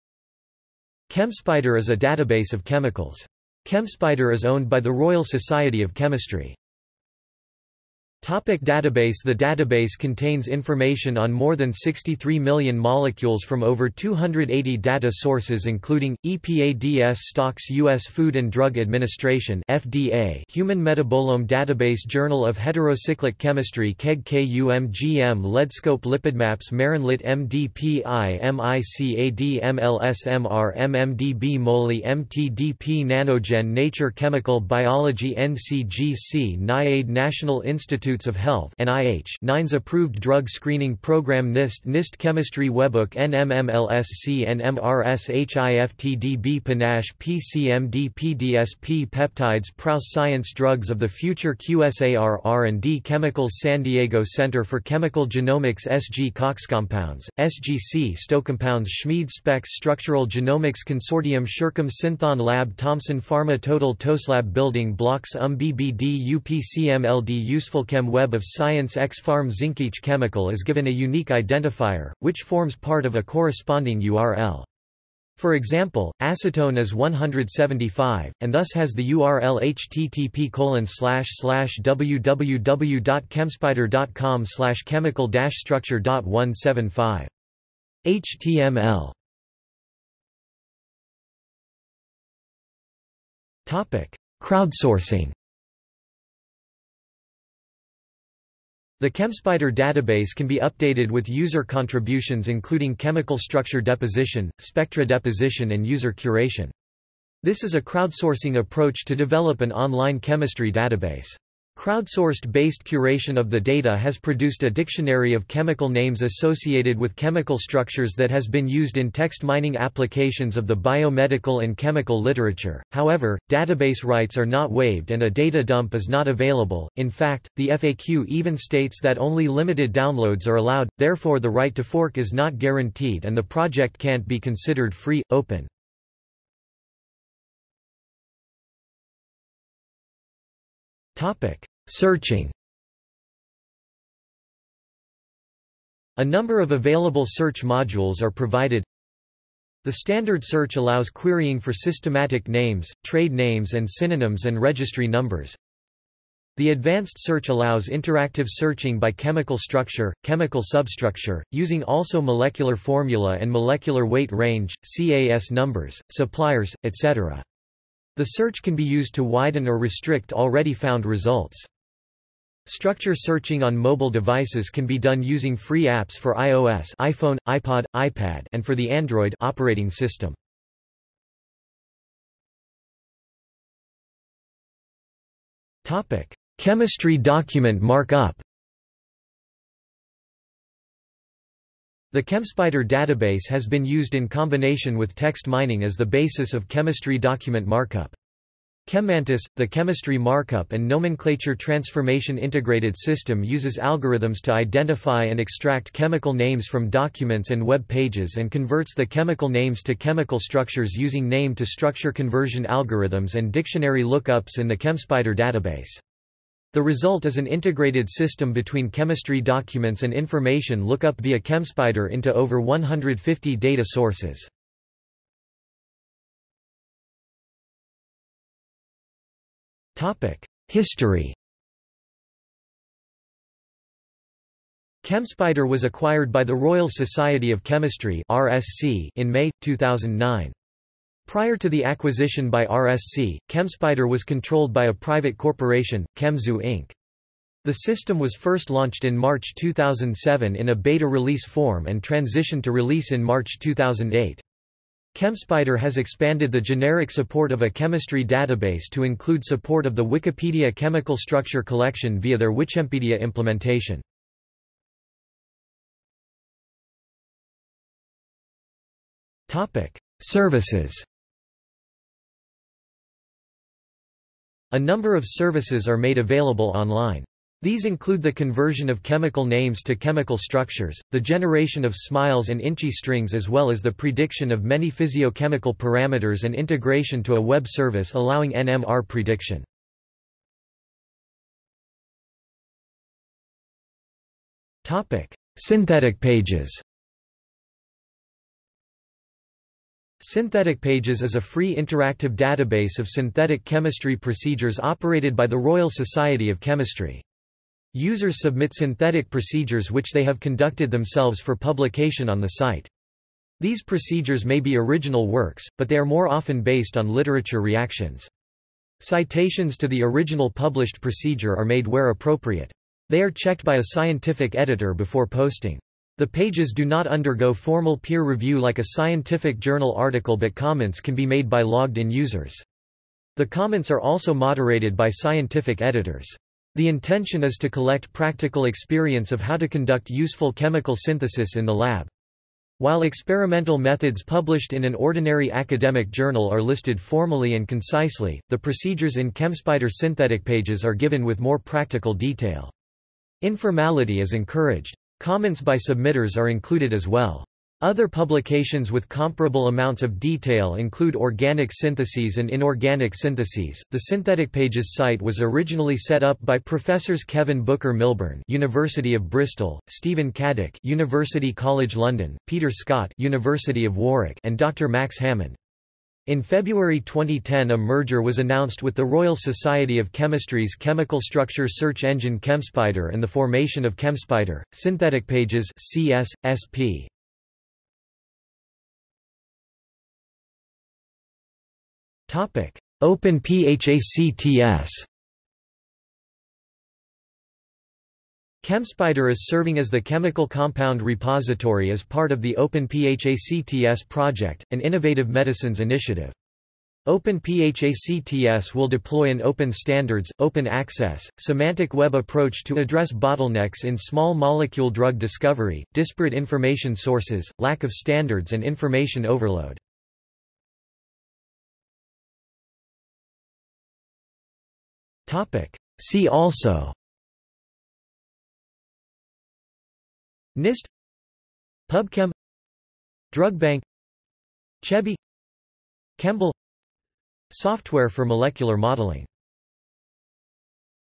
ChemSpider | Wikipedia audio article
00:00:08 1 Database
00:01:30 2 Crowdsourcing
00:02:17 3 Searching
00:03:02 4 Chemistry document mark-up
00:03:46 5 History
00:04:25 6 Services
00:04:51 6.1 SyntheticPages
00:06:28 6.2 Open PHACTS
00:07:01 7 See also

- Socrates

SUMMARY
ChemSpider is a database of chemicals.  ChemSpider is owned by the Royal Society of Chemistry.